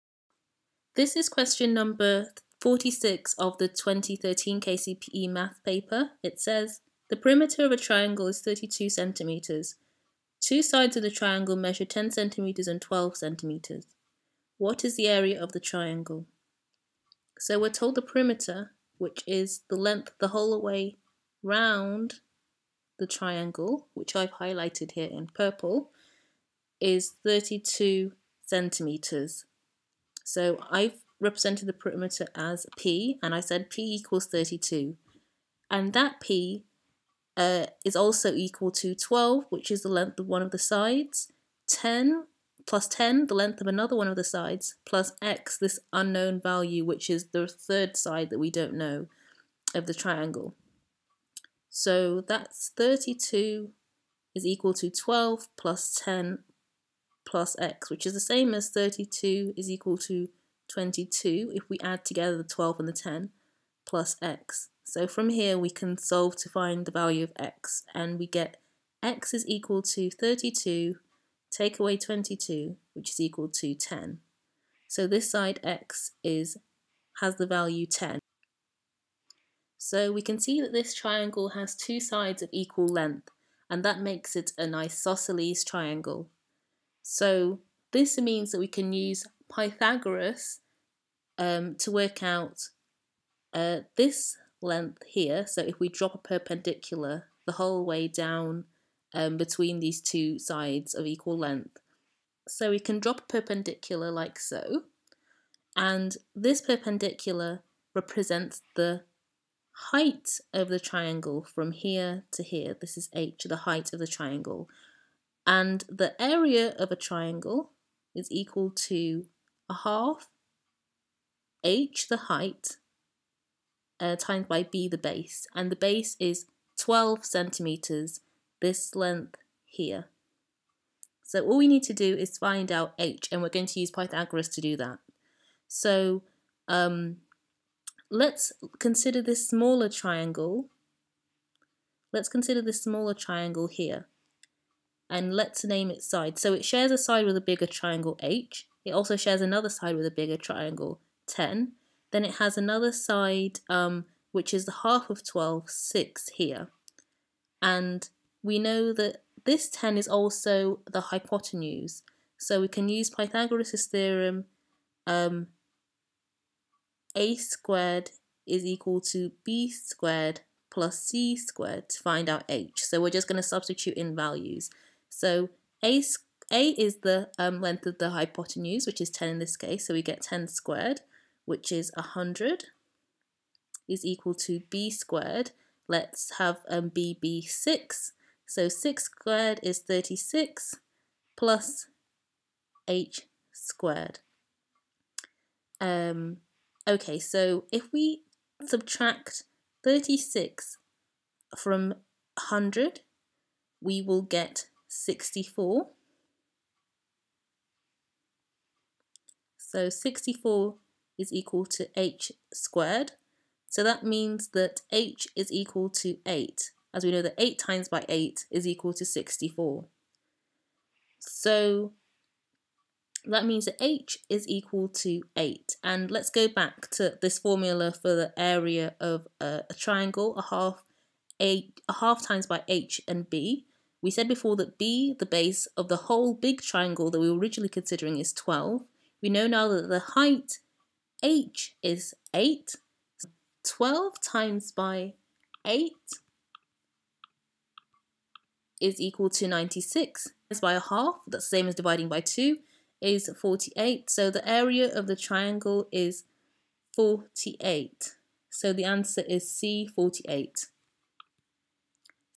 KCPE 2013 Mathematics Paper: Question 46 (Area, Perimeter, Triangles | Kenyan National Exams)
This is the solution to the 2013 KCPE Mathematics Paper. This question concerns the area of a triangle.

We are given the length of two sides of the triangle: one side is 10 cm, the other is 12 cm. We are also given the perimeter which is 32 cm.

The length of each of the 3 sides must equal the perimeter, therefore the length of the third side can be found by subtracting the lengths of the known sides from the perimeter. The length of the unknown side x is therefore 32 - (10 + 12) = 10.

As we have a triangle which two equal sides, it is an isosceles triangle. It has one line of symmetry, and therefore a perpendicular line can be drawn slitting the triangle into two congruent right-angle triangles.

The height of each of these right angle triangles is the same as the height of the original.

We use Pythagoras' Theorem to find the height. a^2 = b^2 + c^2
H = 8 cm. We use this to calculate the area of the original triangle.

Area of triangle = 1/2 * h * b = 1/2 * 8 * 12 = 48 cm ^2.